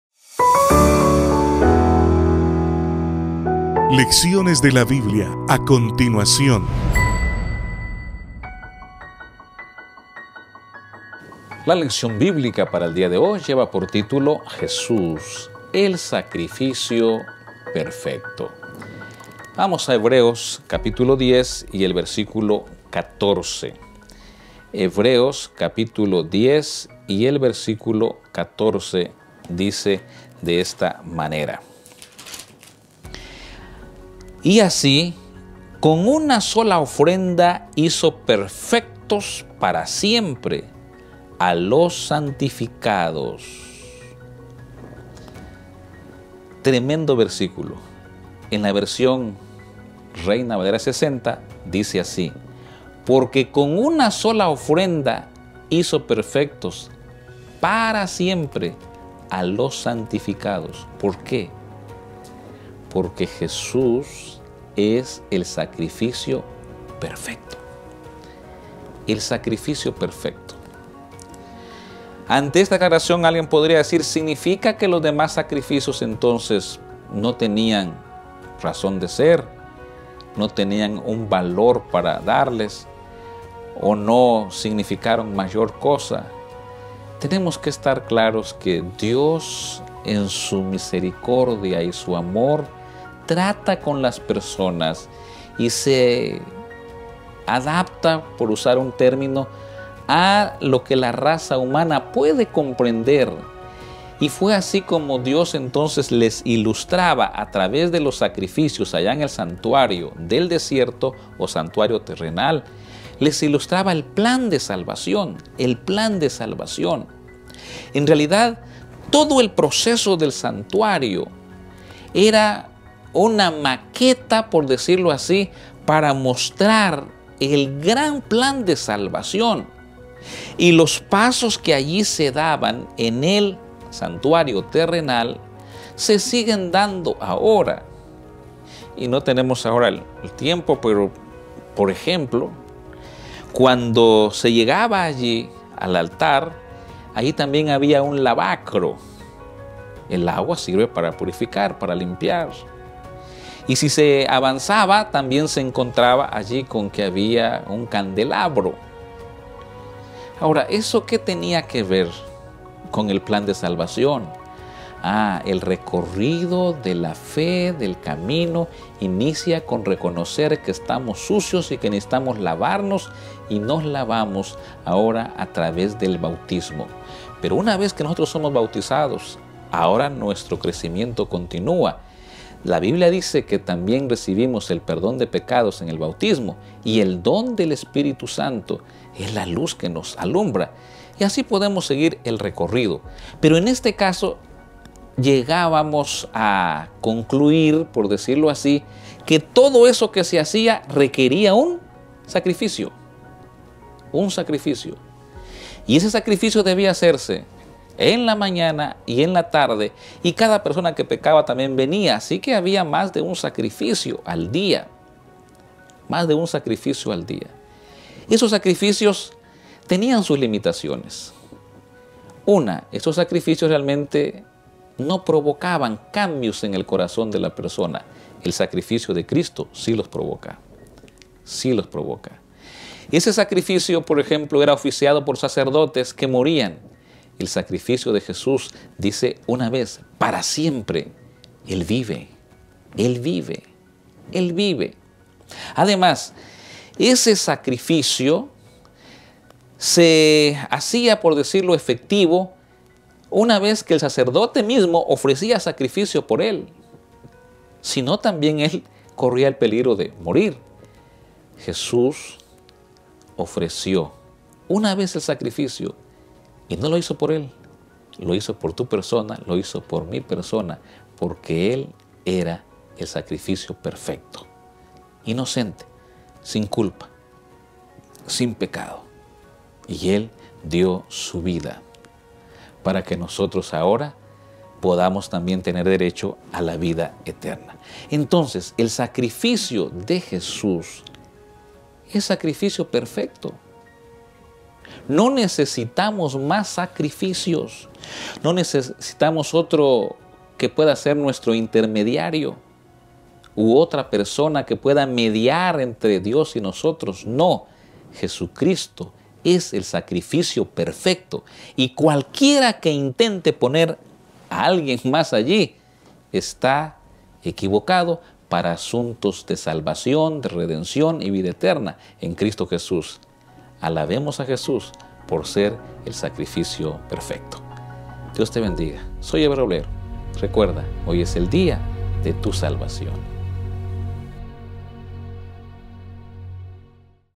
VIERNES 25 FEBRERO - LECCIONES DE LA BIBLIA
La lección de Escuela Sabática para este día se titula: JESÚS EL SACRIFICIO PERFECTO, un tema presentado por el Pastor Ever Roblero.